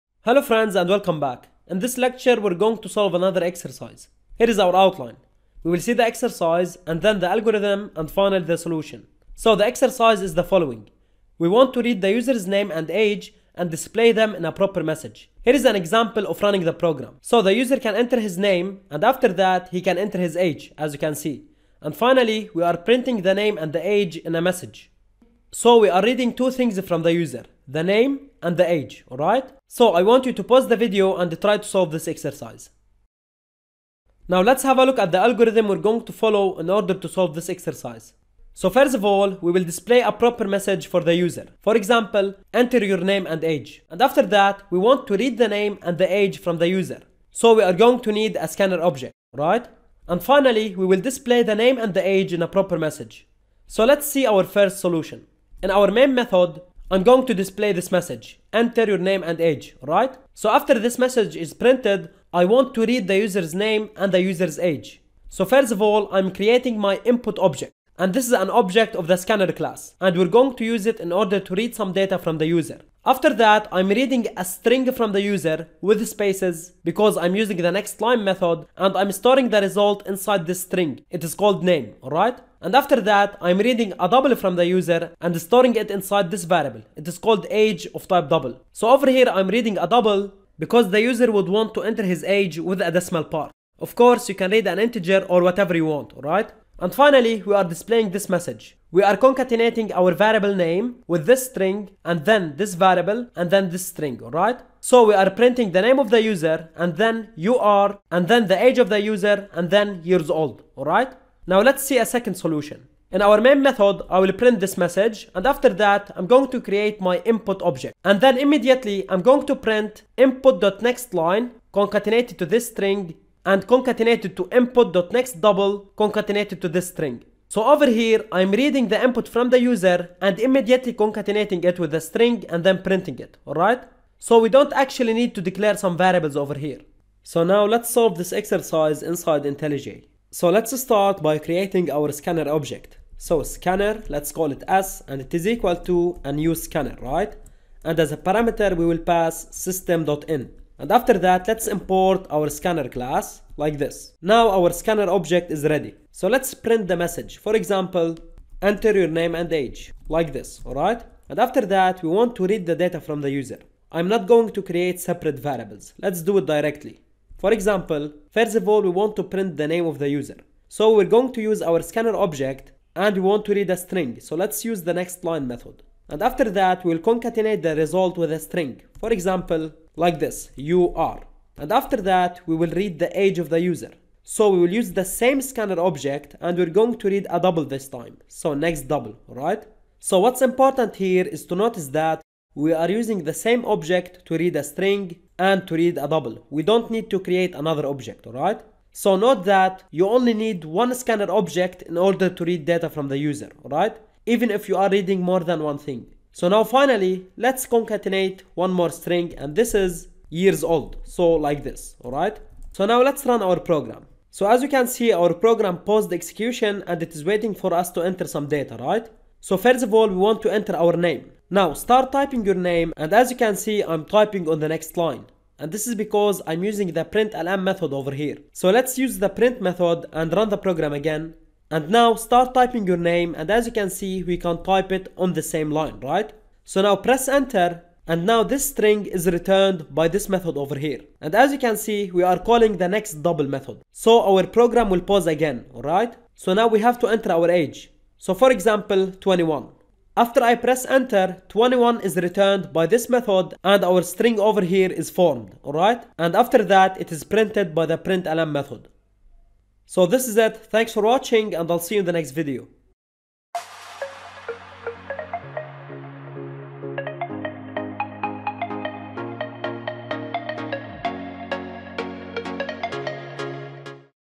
Java Exercise – Name and Age
Java Programming: Name and Age Program in Java Programming
Topics Discussed:
1. Reading multiple values using one Scanner object.